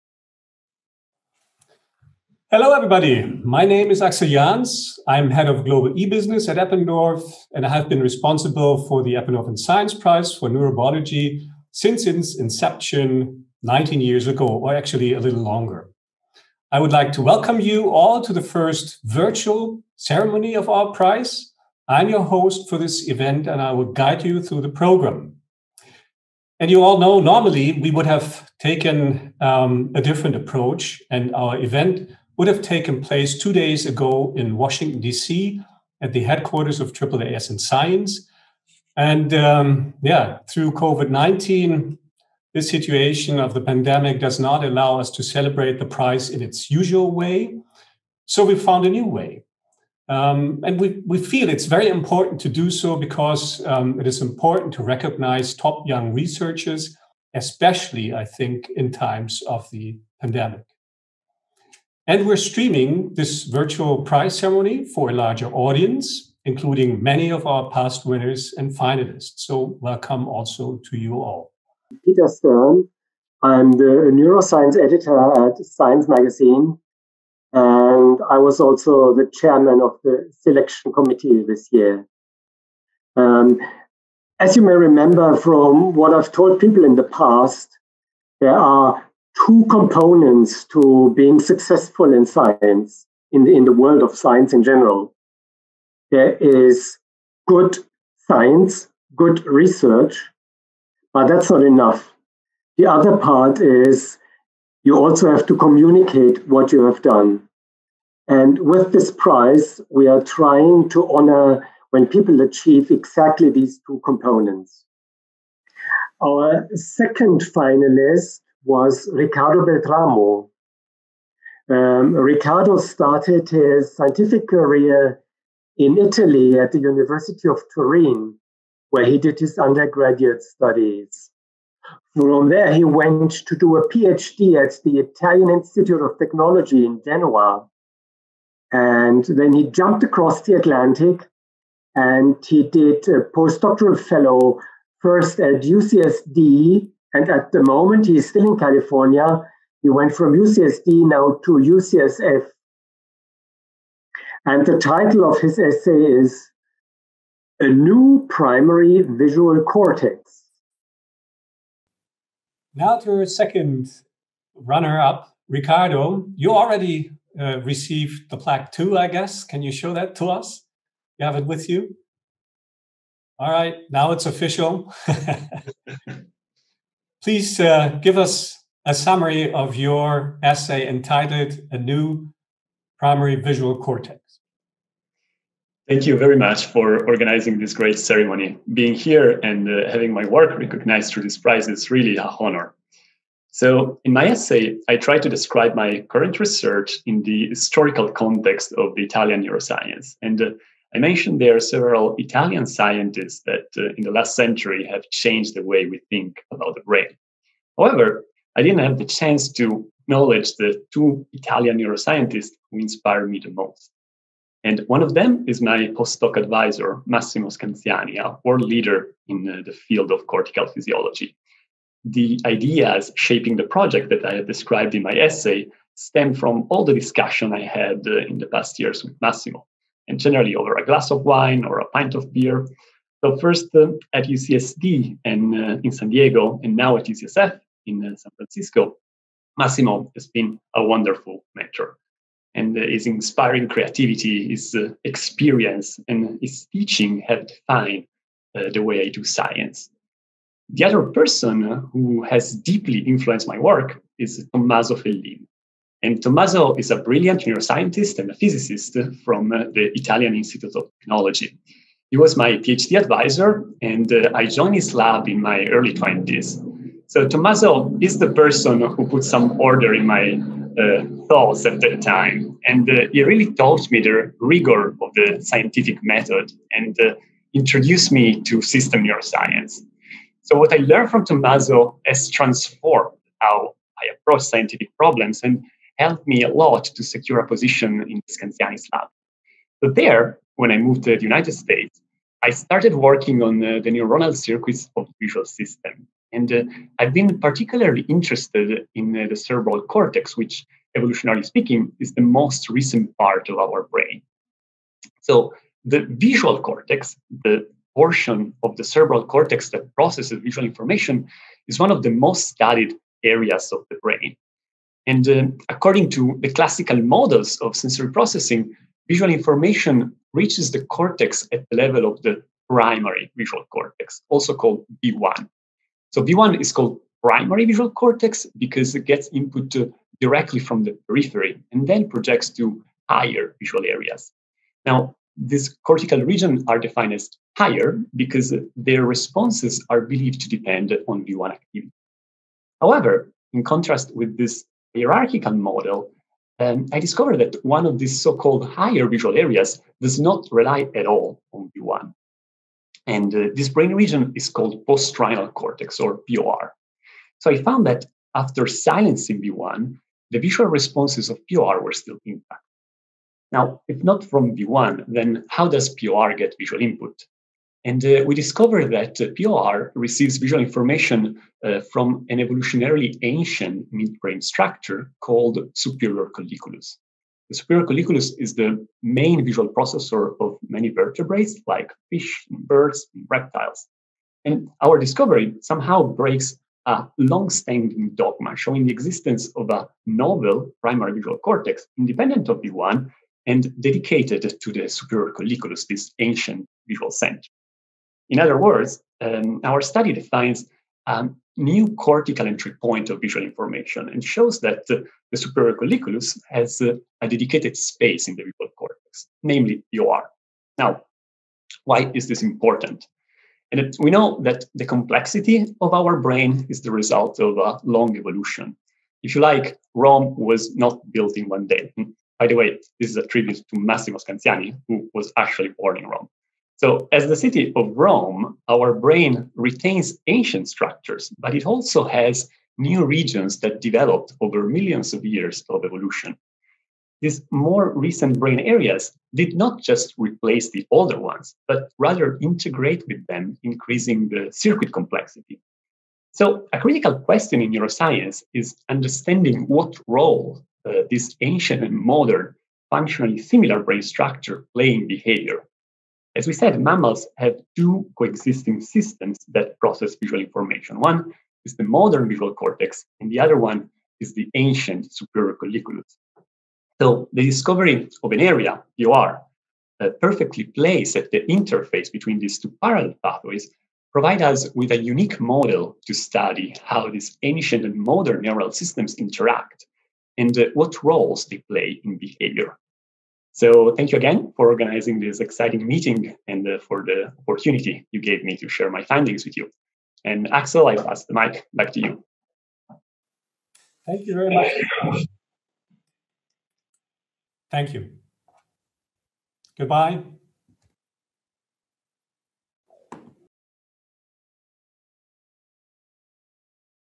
Beltramo Riccardo - "A new primary visual cortex" - Eppendorf & Science Prize (2020)
Riccardo Beltramo received his undergraduate degree from the University of Turin and a Ph.D. from the Italian Institute of Technology. After his doctoral training, Beltramo joined the Howard Hughes Medical Institute at the University of California, San Diego and the University of California, San Francisco, where he is completing his postdoctoral work. He studies sensory perception in the mouse visual system, focusing on understanding how cortical and subcortical neural circuits process visual information to drive behavior.